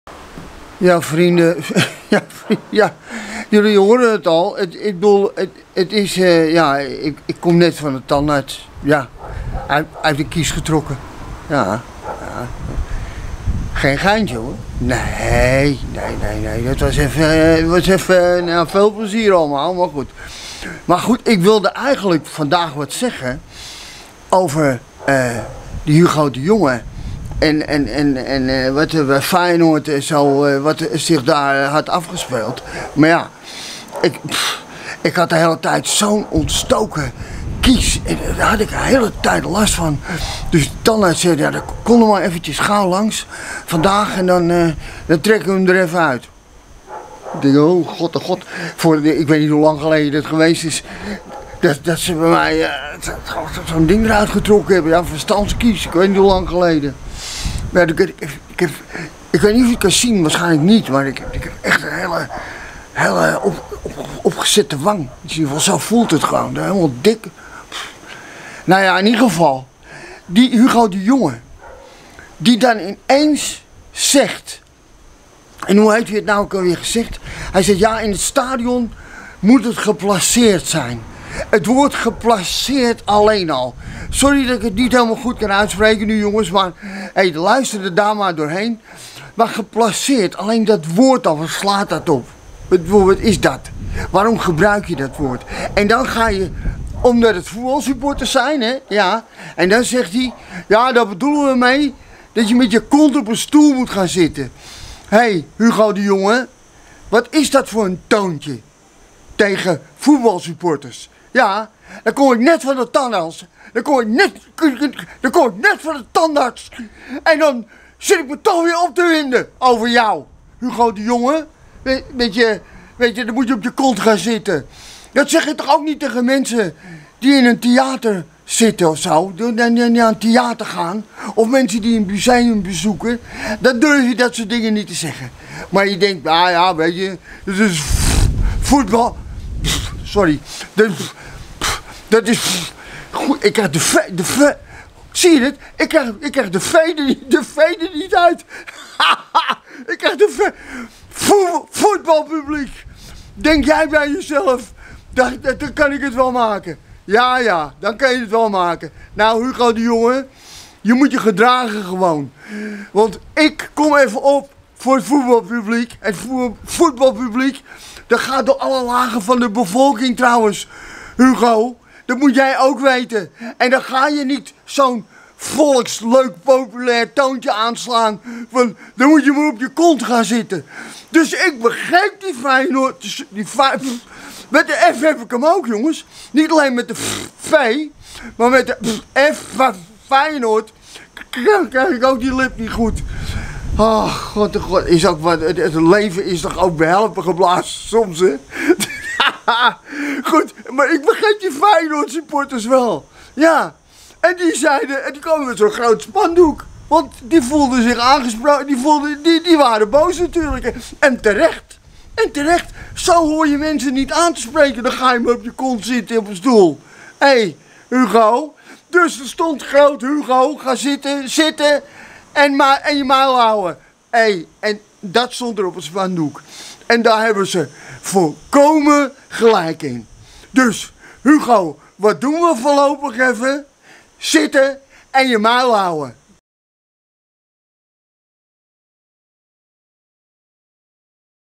Dus Hugo, zitten en muil houden!
Harry krijgt bijna z'n muil niet open, ff bij de tandarts geweest. Maar eigenlijk wilde hij iets zeggen over Hugo de Jonge en z'n muil houden. Dat blijkt toch een beetje ingewikkeld. Kijk maar!!